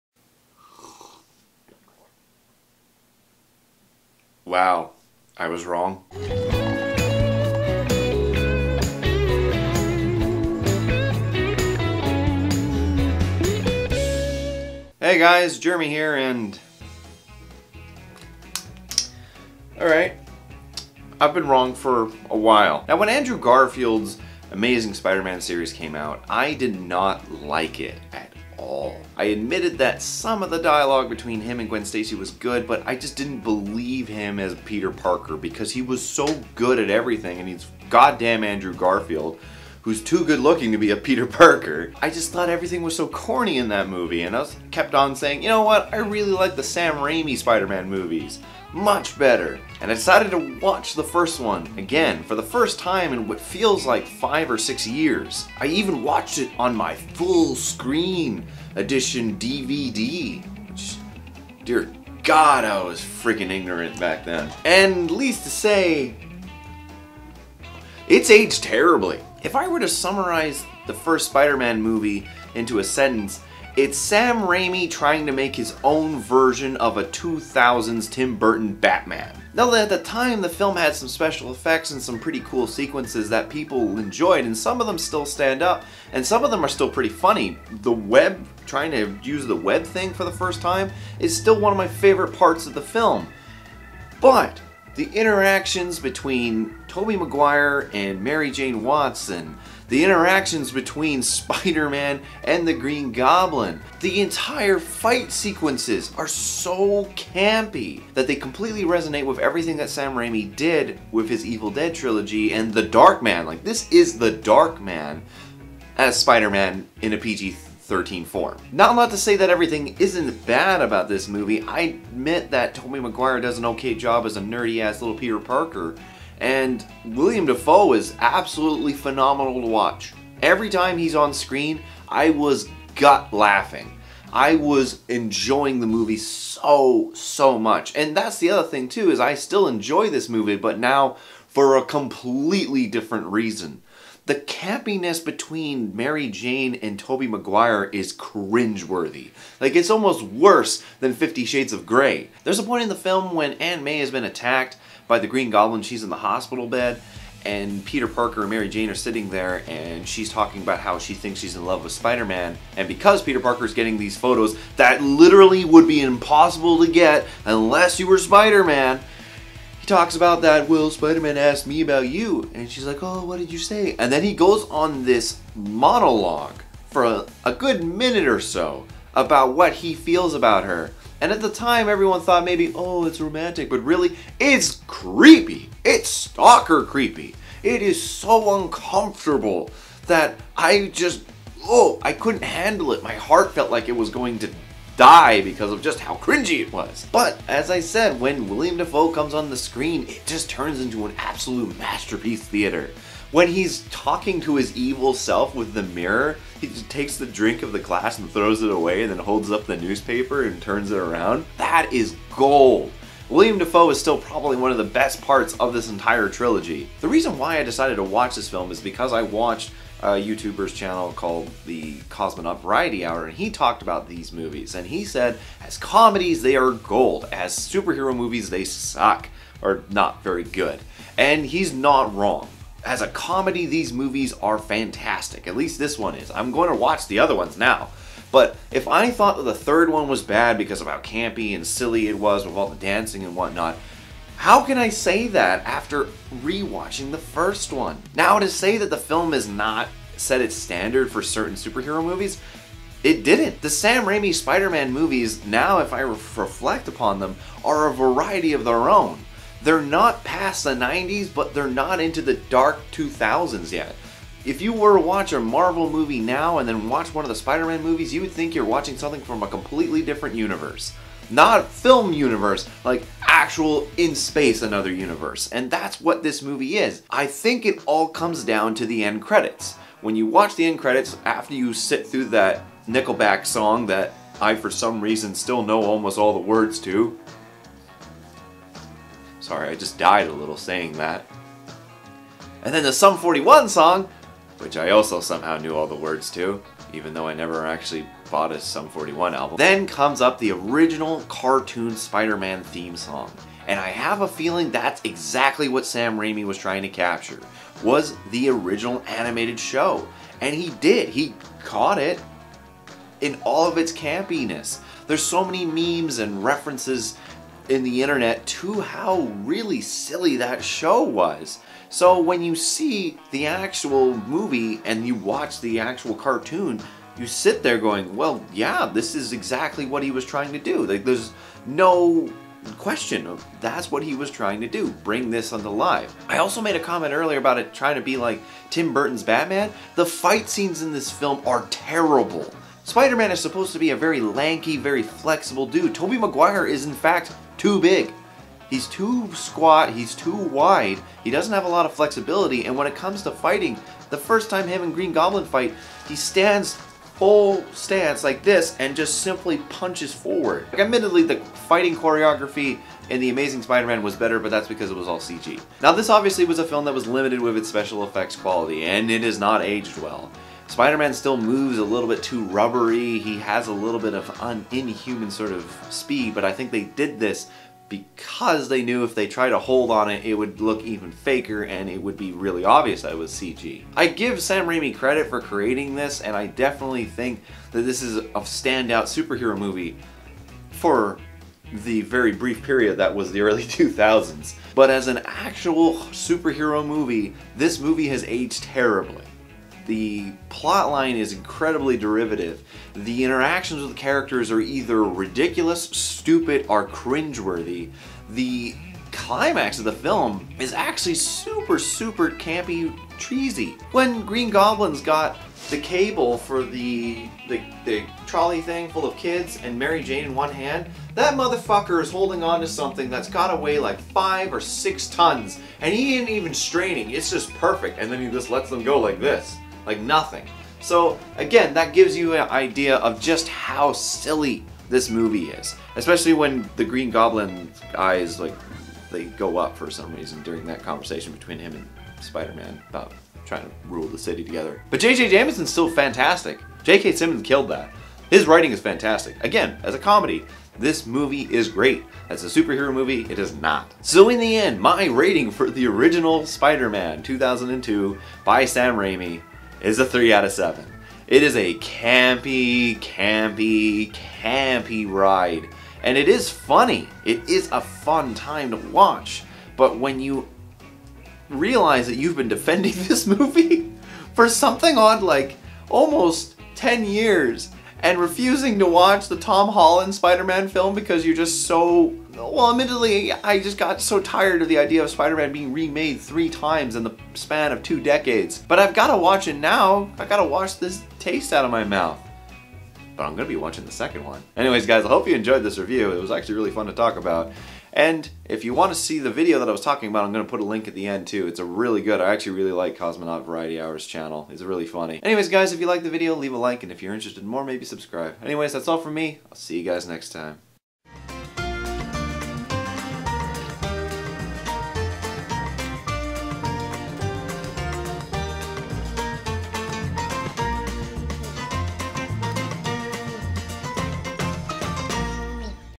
Spider-Man Movie Review
So I finally decided to rewatch the Sam Raimi Spiderman movie ... and it's not good as a superhero movie anymore. As a comedy though, it's freaking golden!